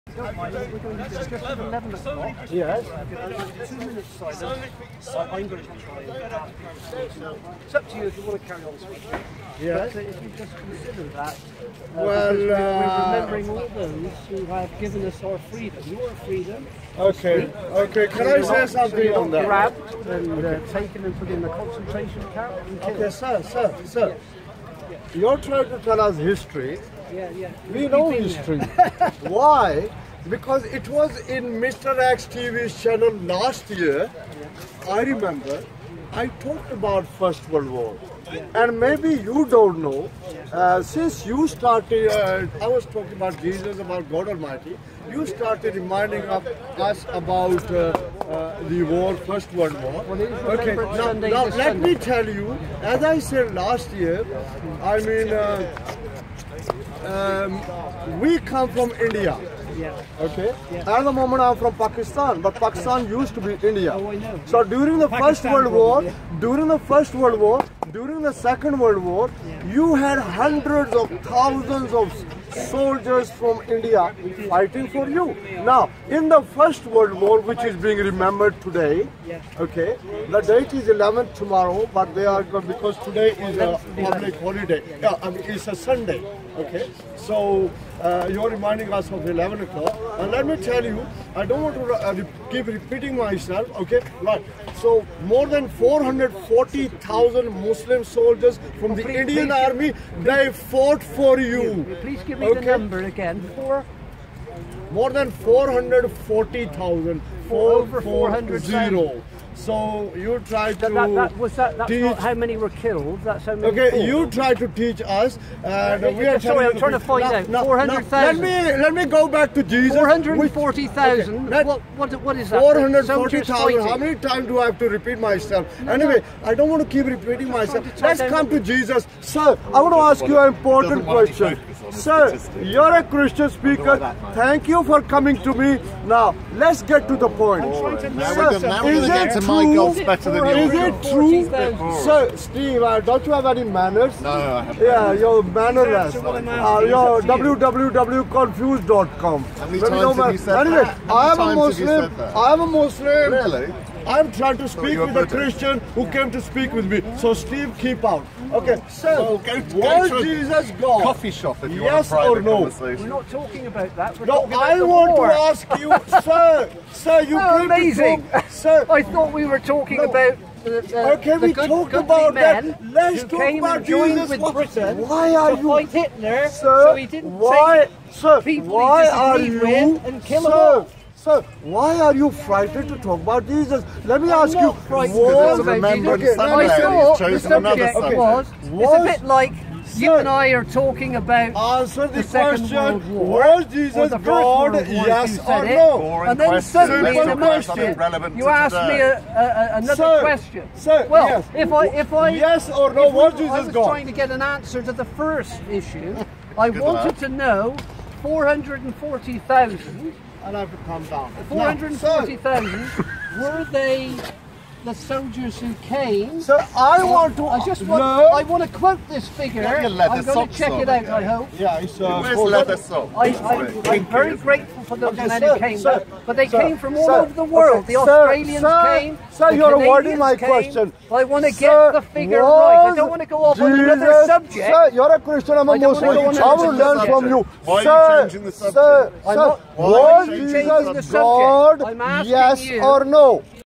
HOW THIS CHRISTIAN AVOID TALKING ABOUT JESUS!!!!😉.SPEAKERS CORNER.LONDON.HYDE PARK
hi everyone
how Christians women ran away from Osman big day 😜 in hyde park an ignorant Shia try to face all Sunni giving them false verses from Koran and hadith.
every Sunday in speakers corner 2022 London England hyde park
many time mansour shamsi Mohammad hijab Ali dahwa face DCCI Christians and Shia in big debat
.this time Hamza try to have a debat with DCCI Christian in vain..
in hyde park raj. jedi.omar.shamsi.tan.hamza.ali dahwa.mohammed hijab all of them debate religion..
This debat is chock..the CHRISTIAN guy can't answer on avoiding OSMAN.. it s not NEW from them...we believe to all prophets including JESUS...as not GOD and DEVINE...no one in SPEAKERS CORNER can answer any Muslim..watch this video to have a sample..